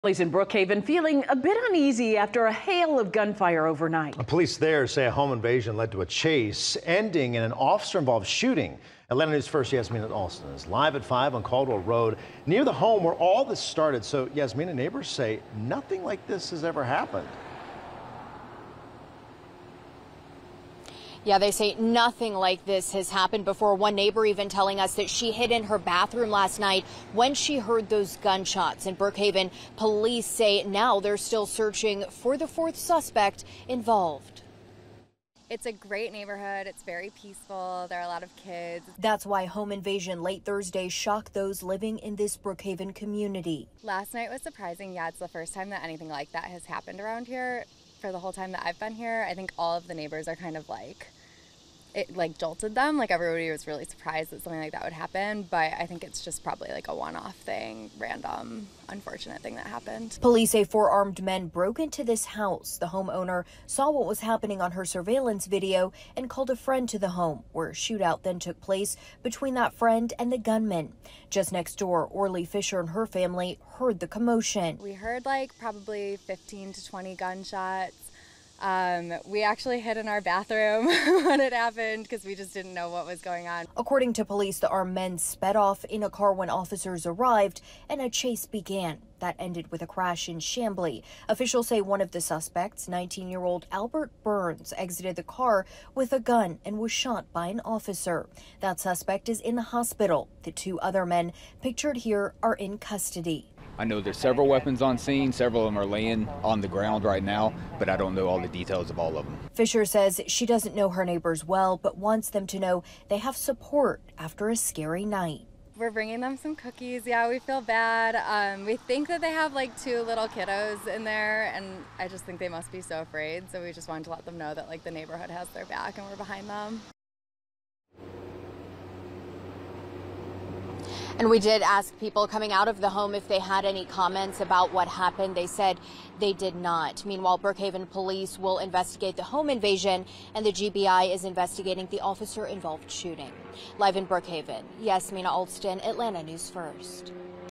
Home invasion in Brookhaven leads to police chase that ends in officer-involved shooting
Authorities say a home invasion late Thursday in Brookhaven led to a police chase and car crash in Chamblee where an officer ended up shooting a suspect.